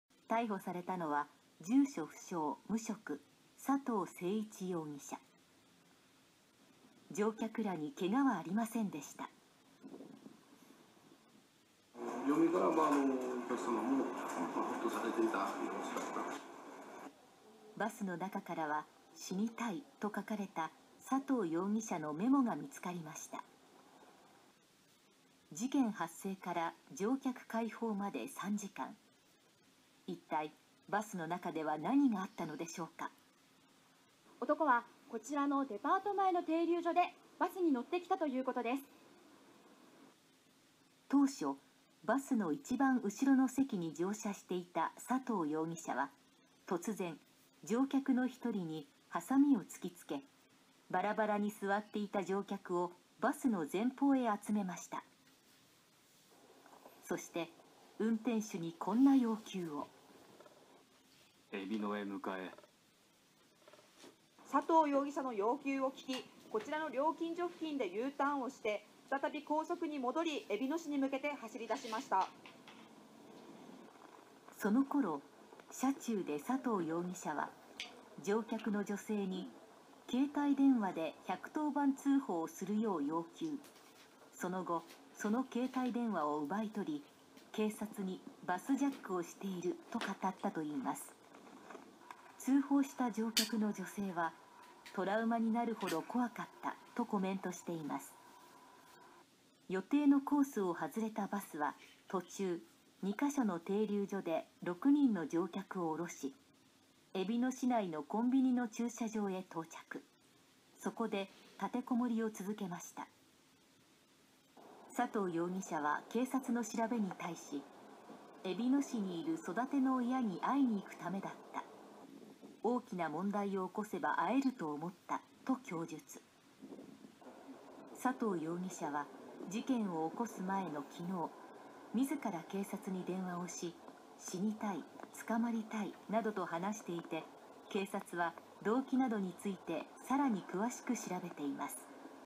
宮崎でバスジャック！男を確保！乗客乗員にけがなし
１１日午後９時過ぎ、宮崎交通（本社・宮崎市）の宮崎駅発西都城駅行き特急路線バスが、男に乗っ取られた。バスは宮崎県えびの市のコンビニエンスストア駐車場に停車し、男は運転手と乗客計４人を人質にして立てこもっていたが、１２日午前１時過ぎ、県警が男の身柄を確保し、監禁容疑で現行犯逮捕した。県警によると、男は４０歳代で、はさみのようなものを所持していたという。けが人はなかった。
県警によると、バスは宮崎自動車道を走行してえびのインターチェンジ（ＩＣ）を降り、同ＩＣから南東に約２５０メートル離れた「セブン―イレブンえびのインター店」（えびの市永山）に向かった。途中で乗客数人が降ろされ、このうち女性１人が１１日午後１０時過ぎ、「男にバスジャックされた」とする趣旨の内容で１１０番したという。
同店の男性店長によると、同日午後１１時２０分頃に警察官が入店し、従業員に「バスジャックが起きているので、客は入店させません。従業員も外に出ないでください。今から説得作業を行います」と言われ、バスジャックの発生を知った